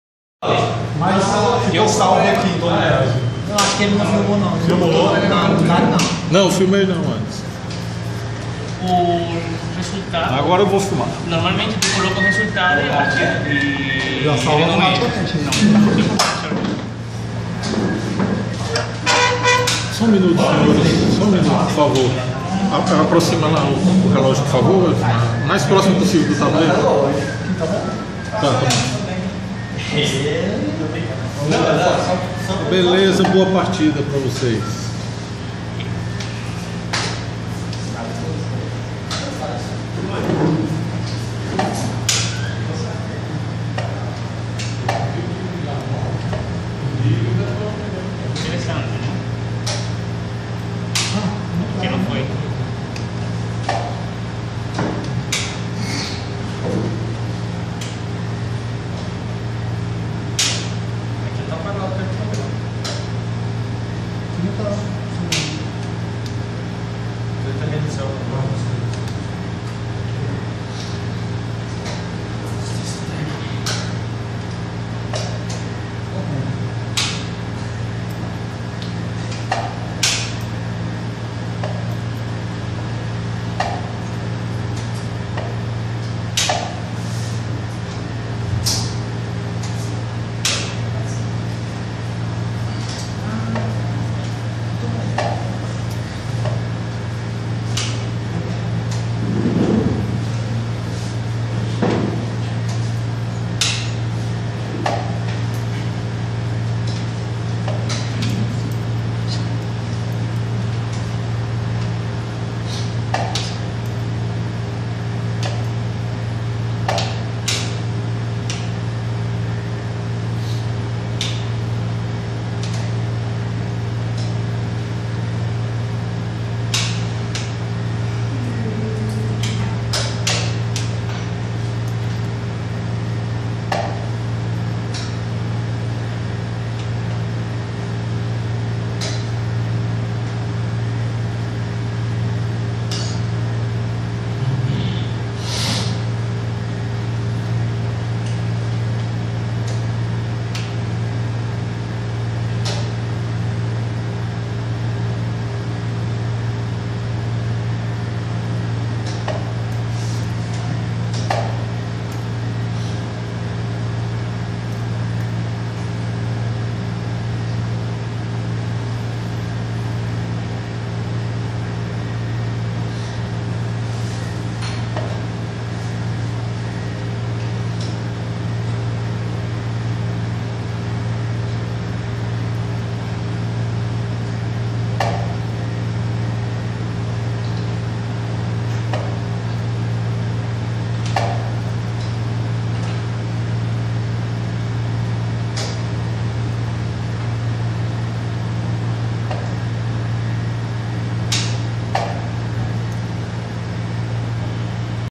♚ Eudismar Abreu vs GM Neuris Delgado ☆ Rápidas com o Grande Mestre ☆ 2015
♘ Ficha do Evento ♘

Ritmo: 05m+03s em 10 rodadas simples

Local: Clube Teresinense de Xadrez - CTX

O Clube Teresinense de Xadrez - CTX fica localizado na
Av. Dom Severino, nº 1480, 3º andar, em Teresina - Piauí - Brasil.

Dúvidas ou sugestões passem e-mail ou publiquem no grupo do CTX no Facebook.

Vídeos dos eventos no Clube Teresinense de Xadrez